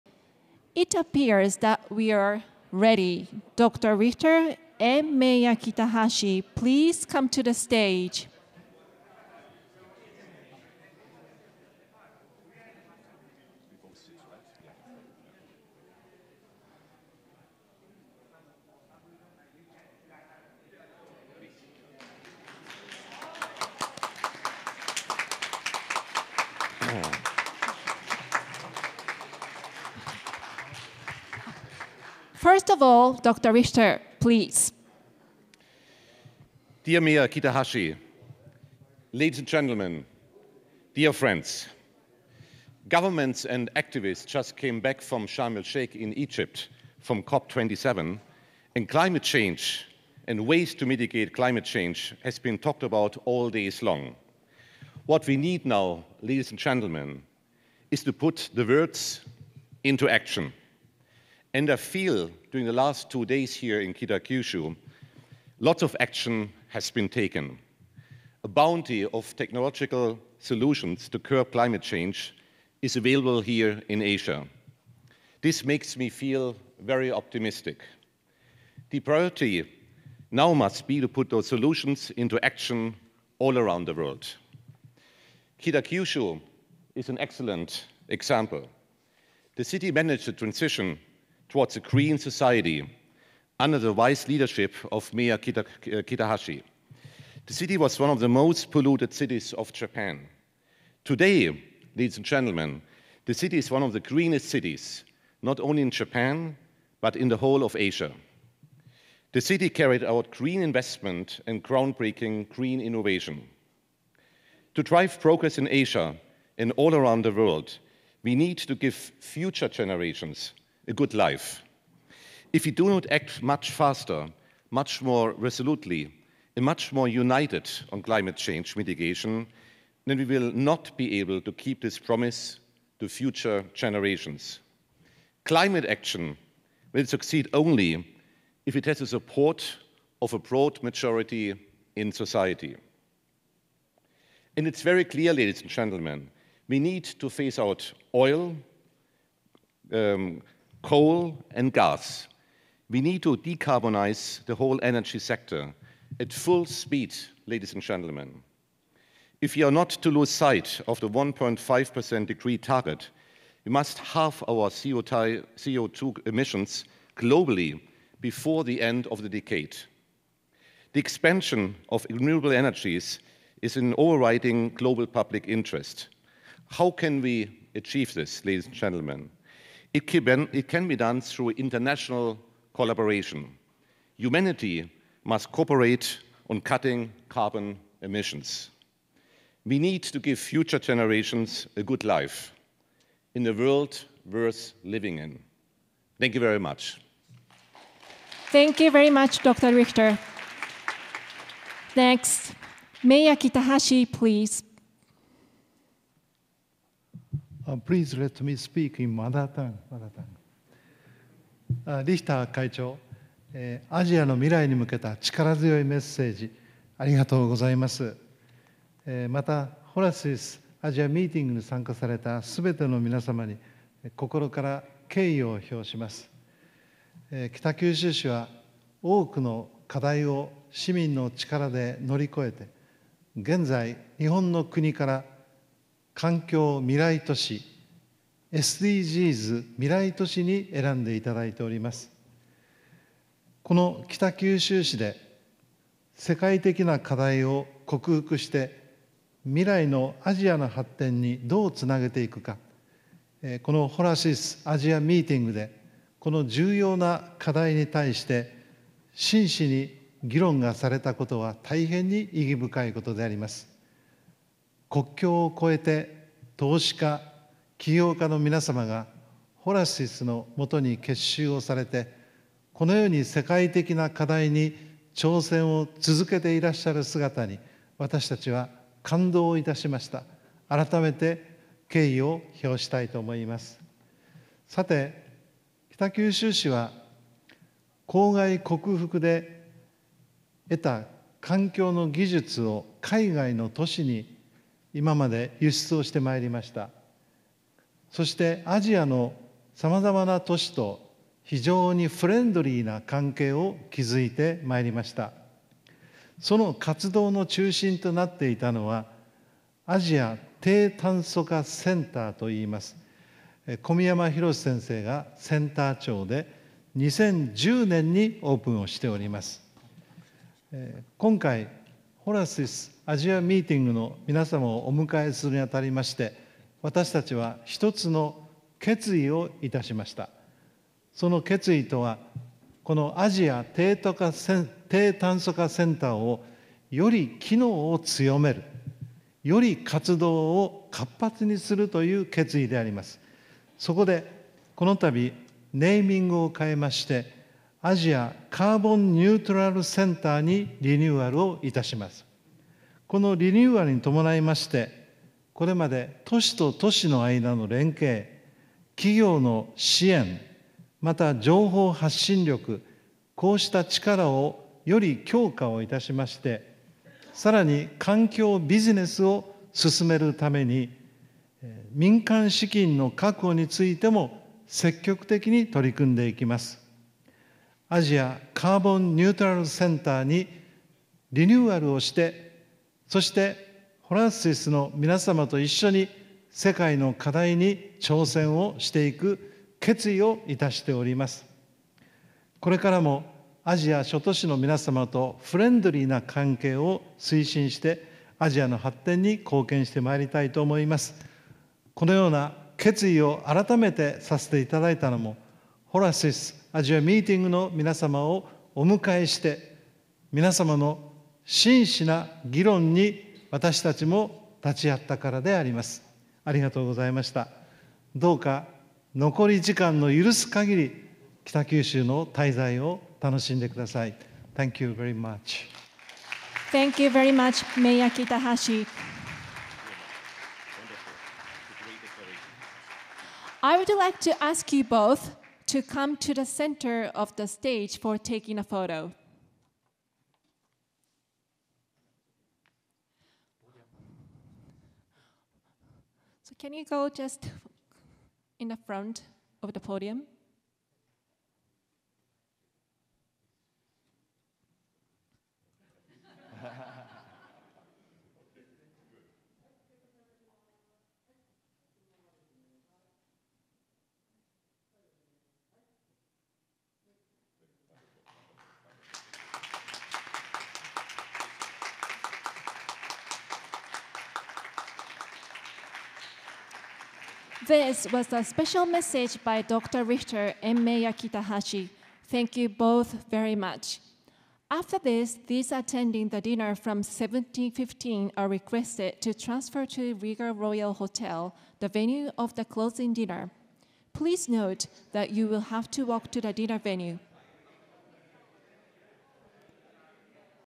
Special Message: Towards the Realization of a Green Society Horasis Asia Meeting 2020
The people of the globe, while mired in many issues, are waking up to the real issues of climate change and the need to meet all the goals of the SDGs. Mayor Kenji Kitahashi and Horasis Chairman Frank-Jürgen Richter share their philosophy on the realization of a Green Society.
• Kenji Kitahashi, Mayor of Kitakyushu, Japan
• Frank-Jürgen Richter, Chairman, Horasis, Switzerland